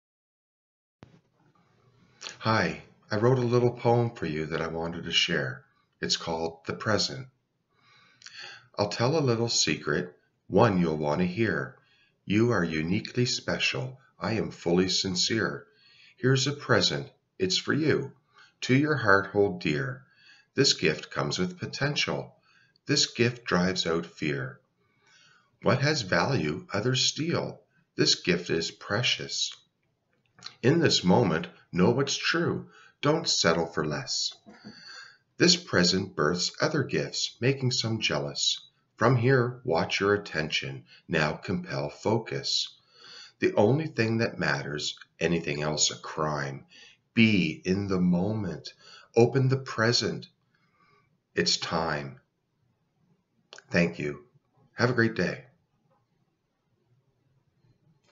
The Present - Poem By Jason William Carr (Encouraging / Inspirational)
The Present - Poem By Jason Carr

I'll tell a little secret,
One you'll want to hear,
You are uniquely special,
I am fully sincere.

Here's a present, it's for you,
To your heart hold dear,
This gift comes with potential,
This gift drives out fear.

What has value others steal,
This gift is precious,
In this moment know what's true,
Don't settle for less.

This present births other gifts,
Making some jealous,
From here watch your attention,
Now compel focus.

The only thing that matters,
Anything else a Crime ,
Be in the moment,
Open the present, it's Time.

Jason Carr